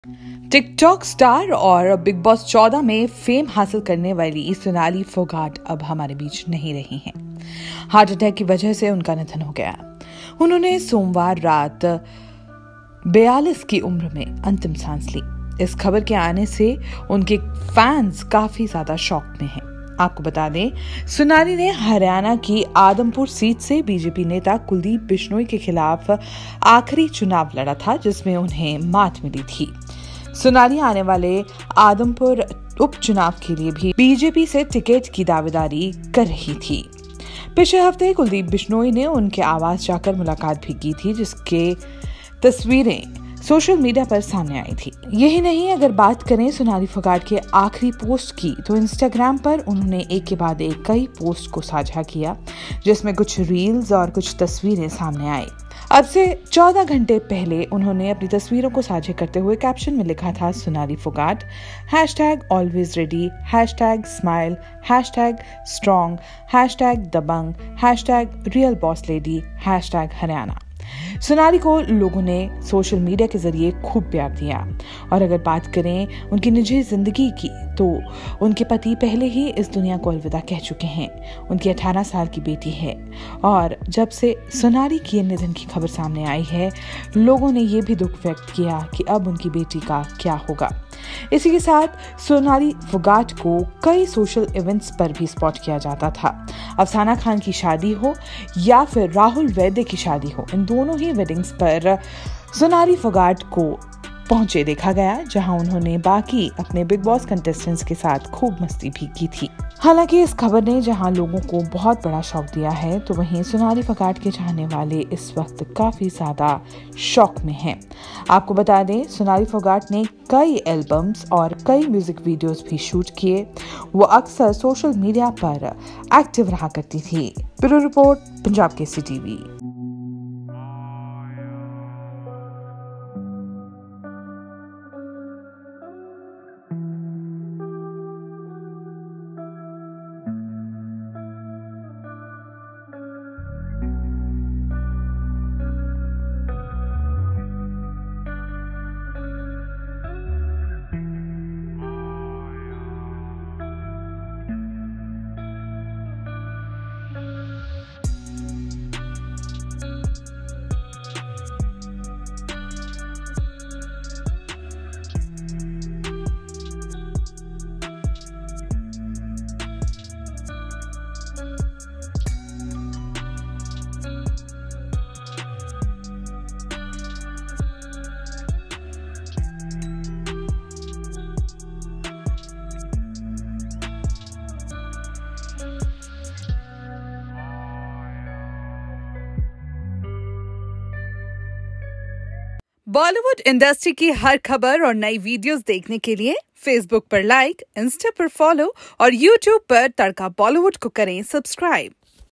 18 साल की बेटी को अकेला छोड़ गई Sonali Phogat, Heart Attack की वजह से हुआ निधन
Big Boss Fame Sonali Phogat is no more, leaves 18 year Daughter Yashodhara Phogat alone.